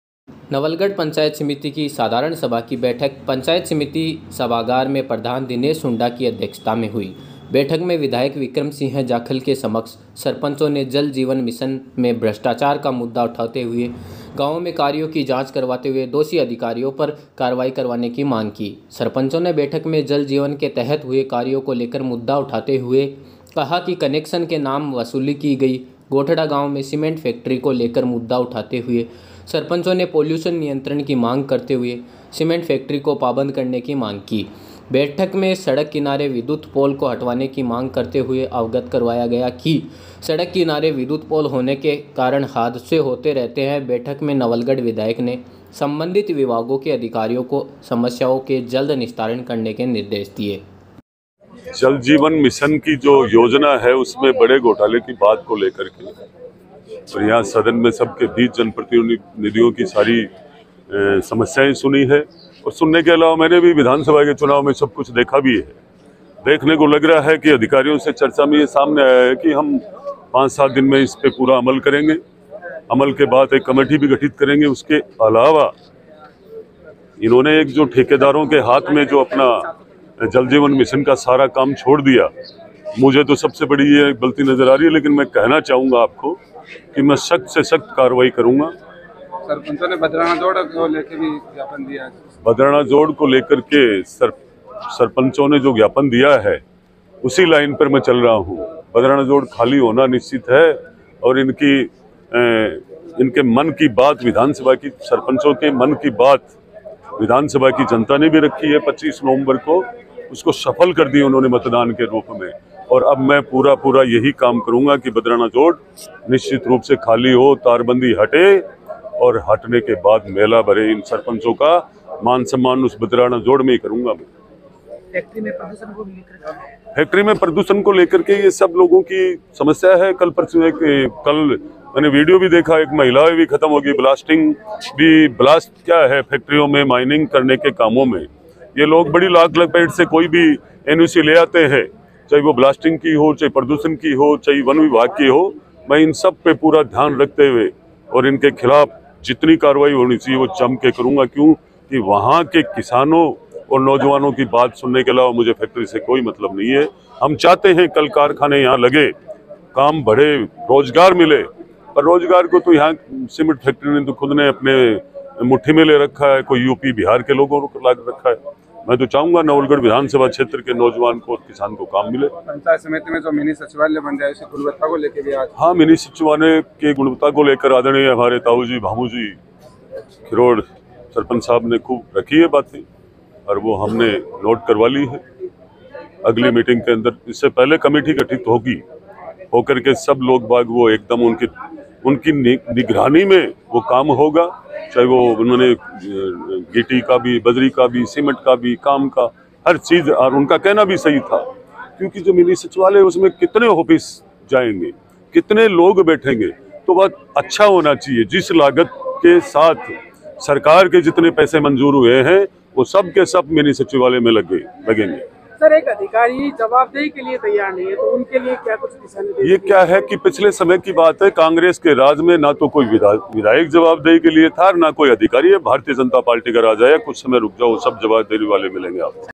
नवलगढ़ : सीमेंट फैक्ट्री से होने वाले प्रदूषण की जांच करवाने की मांग...
झुंझुनूं
नवलगढ पंचायत समिति की साधारण सभा की बैठक
बैठक में सरपंचों ने उठाया जलजीवन मिशन भ्रष्टाचार का मुद्दा
सरपंचों ने सीमेंट फैक्ट्री प्रदूषण की जांच करवाने की मांग
विधायक विक्रम सिंह जाखल ने अधिकारियों को समस्याओं के निस्तारण के दिये निर्देश
प्रधान दिनेश सुंडा की अध्यक्षत में हुई बैठक